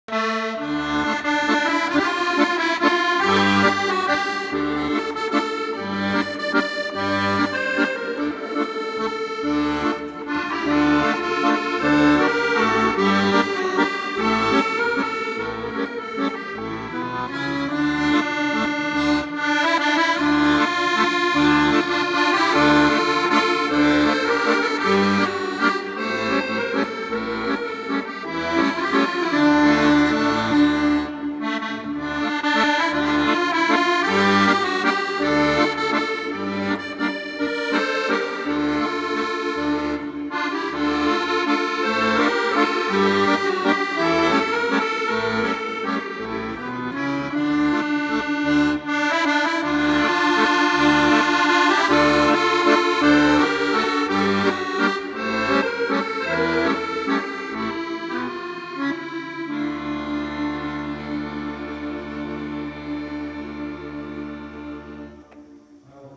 Concierto Navidad Brahojos de Medina
Juan José y su acordeón deleitó a los parroquianos del pueblo con preciosos villancicos como pregón a las Navidades.